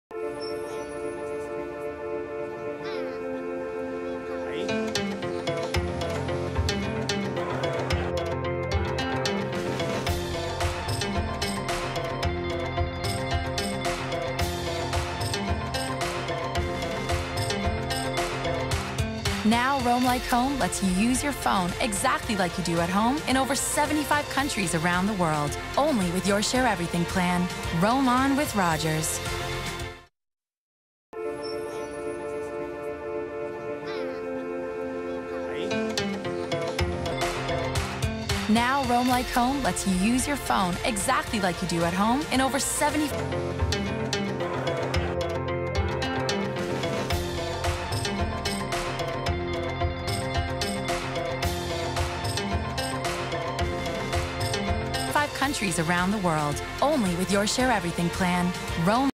Roam Like Home – Worldwide
Rogers CEO Guy Laurence explains how the new “Roam Like Home” service will help snowbirds keep roaming charges to a minimum.

Now you can post your way across Europe with Roam Like Home on your Share Everything™ Plan for just $10/day when roaming in eligible countries. So you .